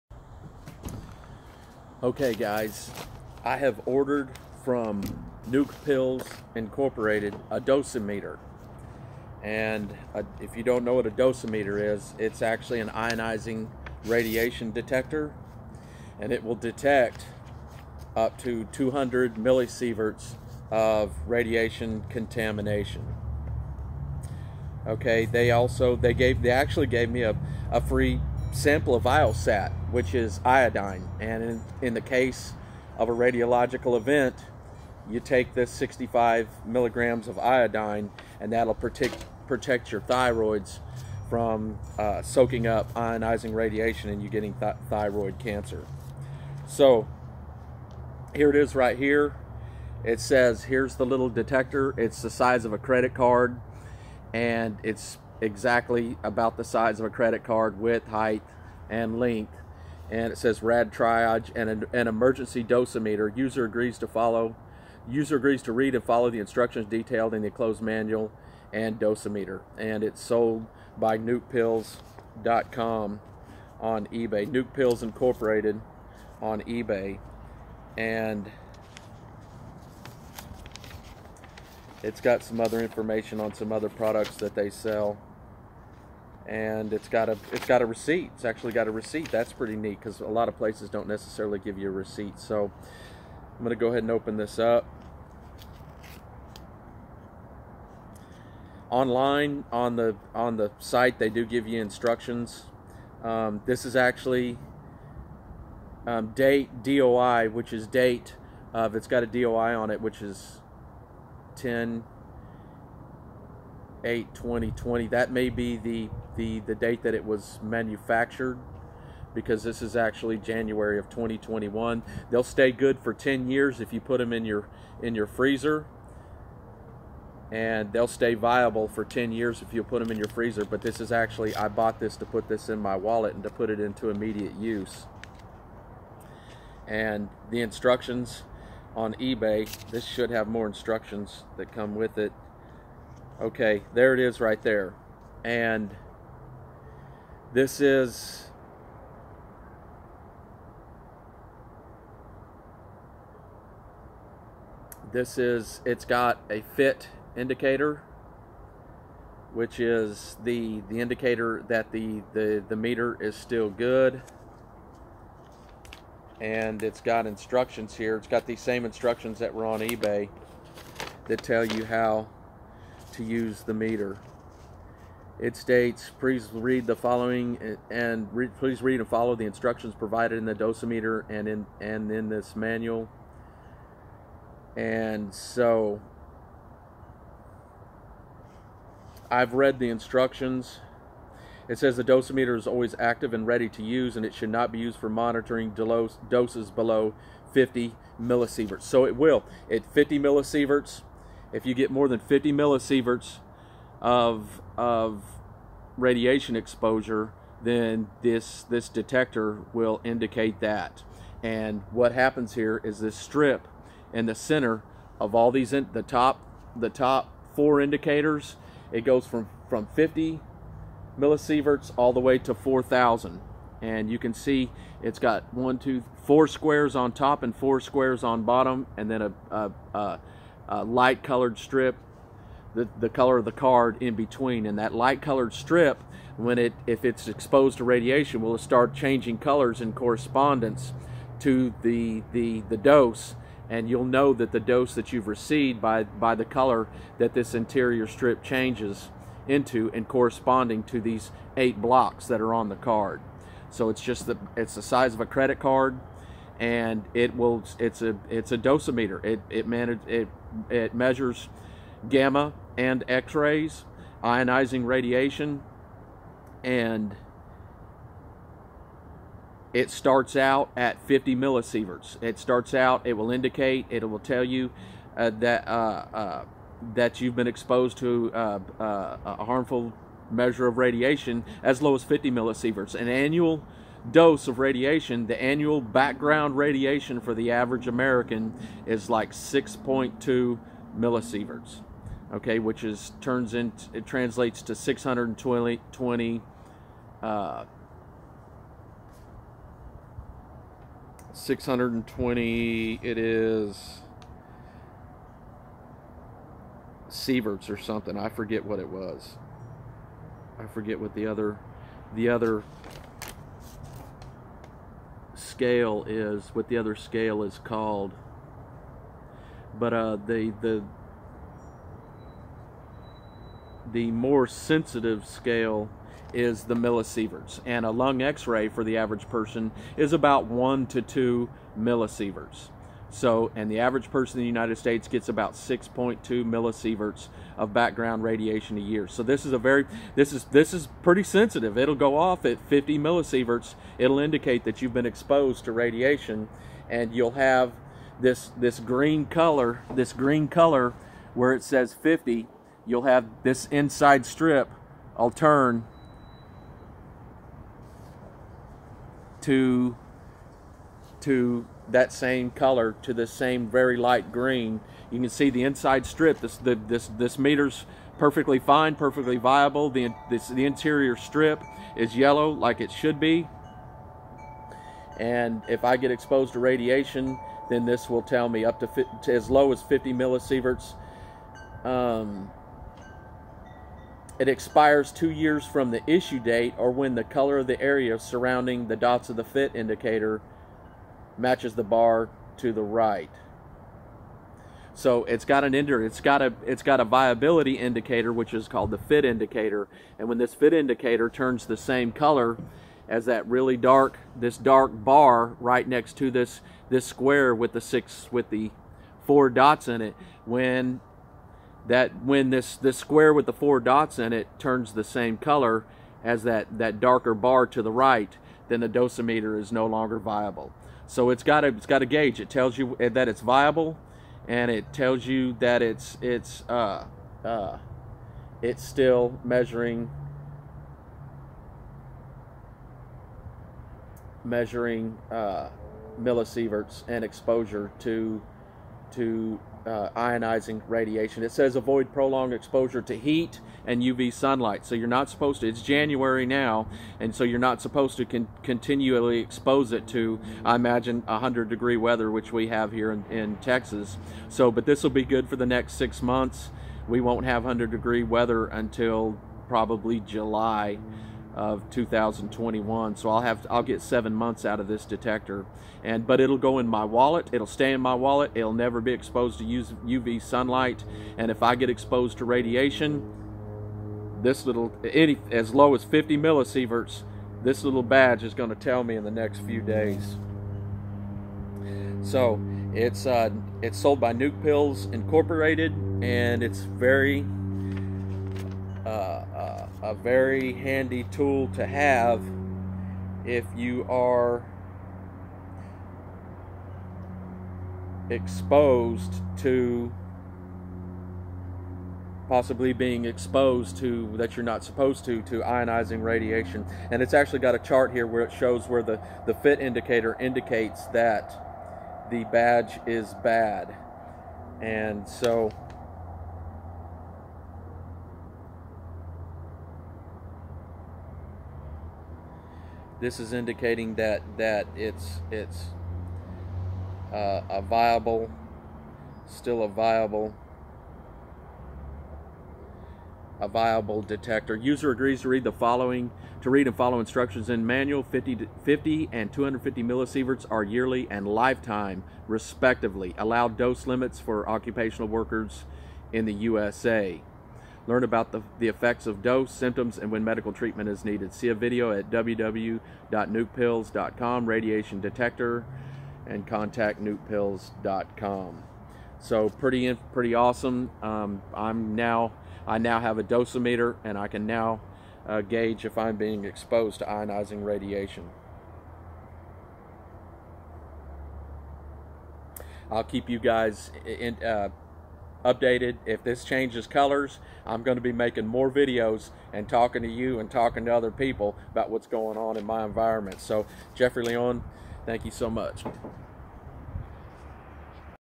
Radtriage personal dosimeter 50 to 4000 millisieverts range and "acute" dosage monitor for radiation
Radtriage personal dosimeter an "acute" dosage monitor that will alert to an exposure to ionizing radiation of 50 to 4000 millisieverts. A real dosimeter that will detect radiation via nuclear bombs, leaking nuclear waste at toxic sites, fallout from nuclear disasters and gamma ray bursts from nearby exploding stars!